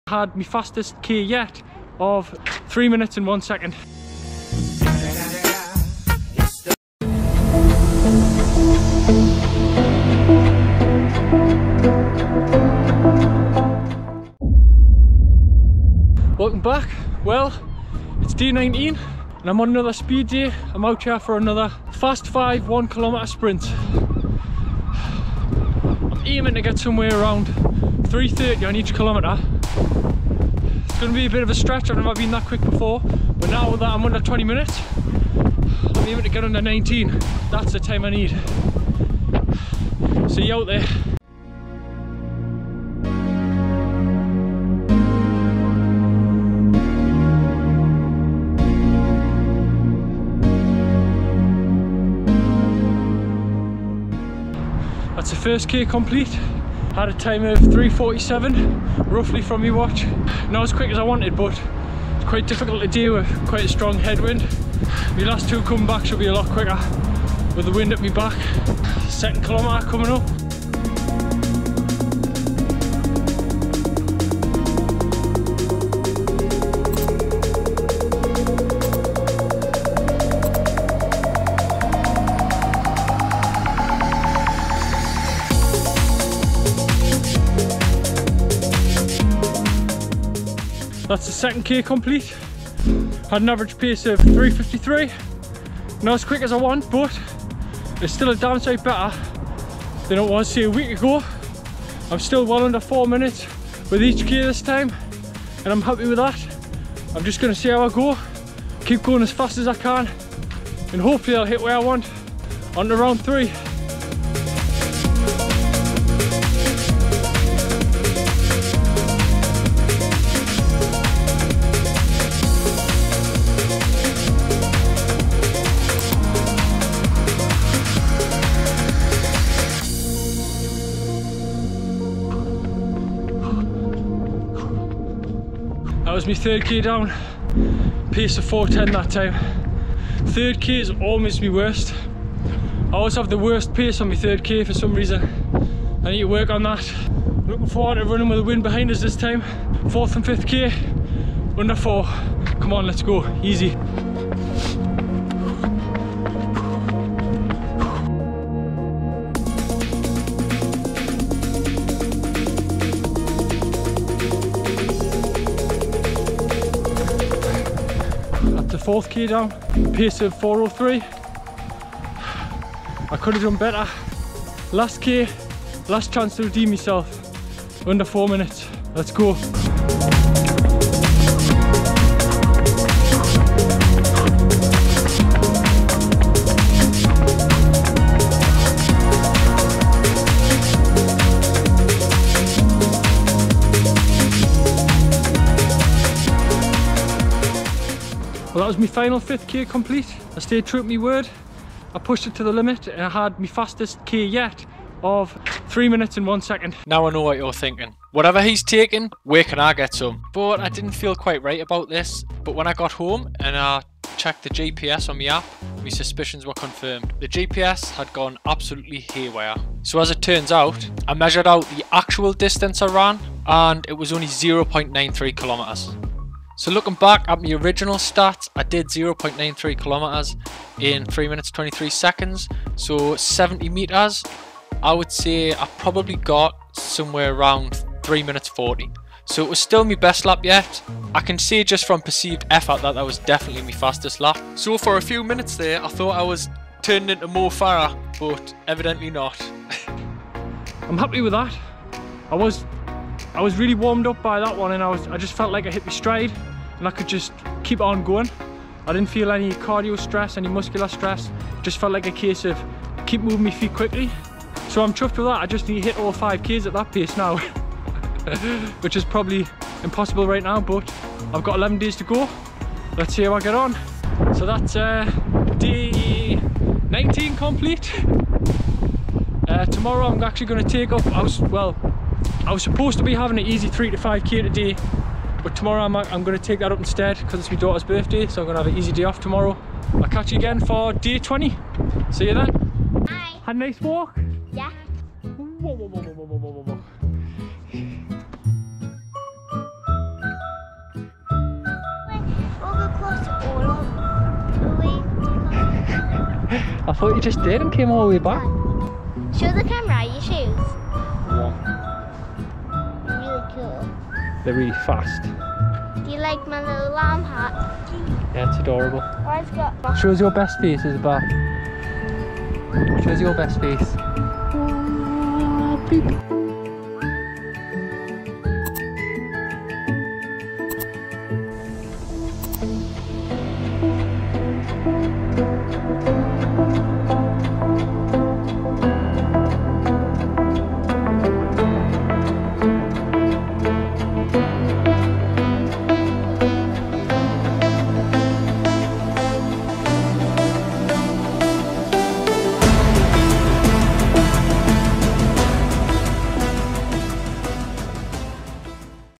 Day 19,did i run a 3 min 1km?? speed day, 5x1k fartlek intervals
Had a good speed day wasn't 100% but still turned out some good times in the circumstance, easy day for day 20.